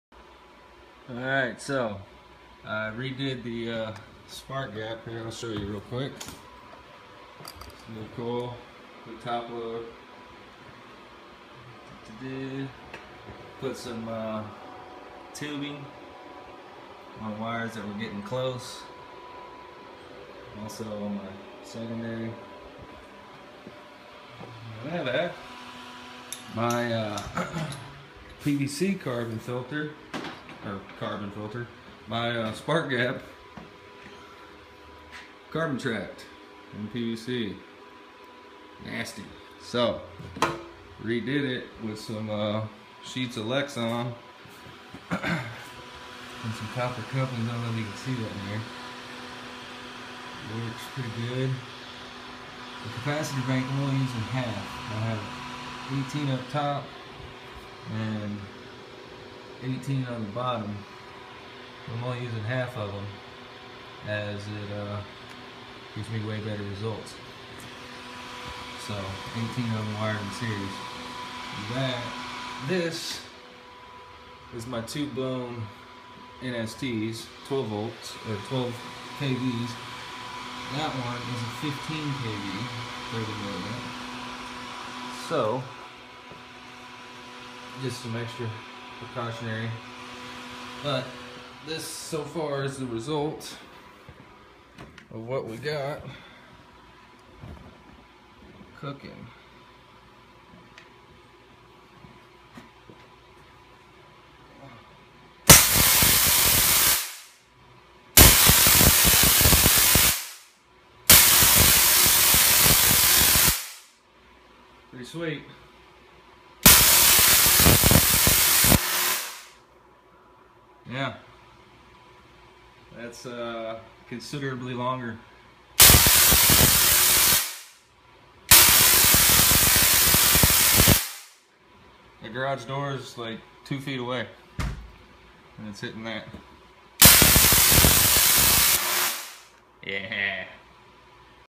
UPDATE: First time tesla coil build part 2. 15kv NST. LONG ARCS
UPDATE: First time tesla cool build part 2. Redid my spark gap to a Richard quick design. It's 6 3/4" copper couplings on lexan sheets. After blowing my two 12kv NSTs (well one side of each) I upgraded to a 15kv 30ma NST. Arcs are way longer than before on just the one 12kv nst.  Not 100% tuned but it's definitely show worthy now. I had an mmc array of 36 caps 18 in each series and then wired the two parallel but I changed it to just running one set of 18. Puts me pretty close to my transformer. Might go up to 21/22 caps which puts me closer. The caps I'm using are the p15k-f I believe. .15uf @ 2kv each.